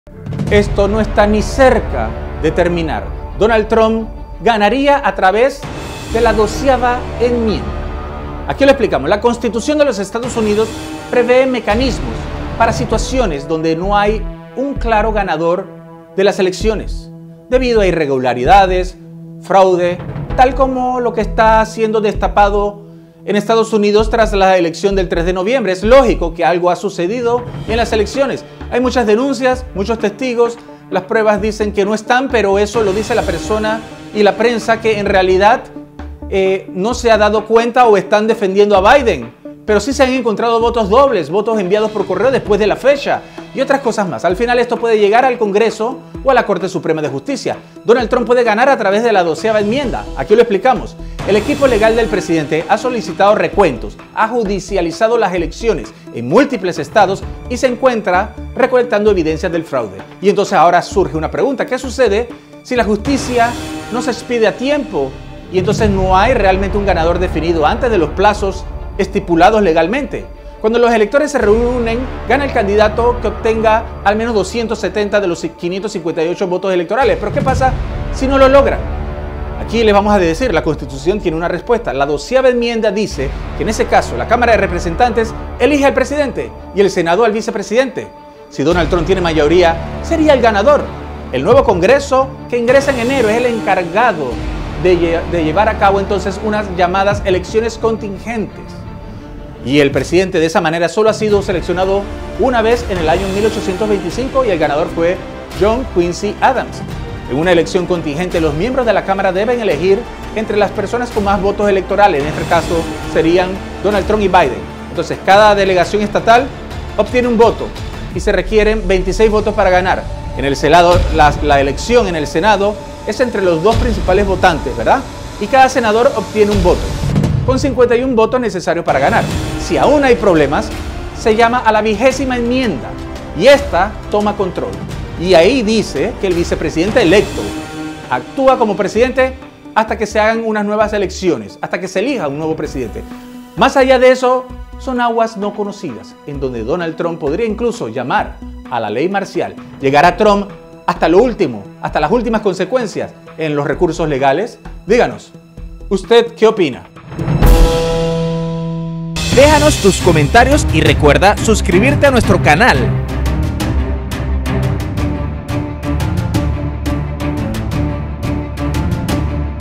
Trump seria reelecto con la 12va enmienda
Trump ganaria a traves de la 12va Enmienda. HOY 7 DICIEMBRE 2020. La Constitución de los Estados Unidos prevee mecanismos para estas  situaciones

Este es un analisis de opinion sobre lo que sucede hoy en EEUU y su relacion con el mundo. Producido por Parecen Noticias Extra

Videos y fotos del Trump Biden by VOA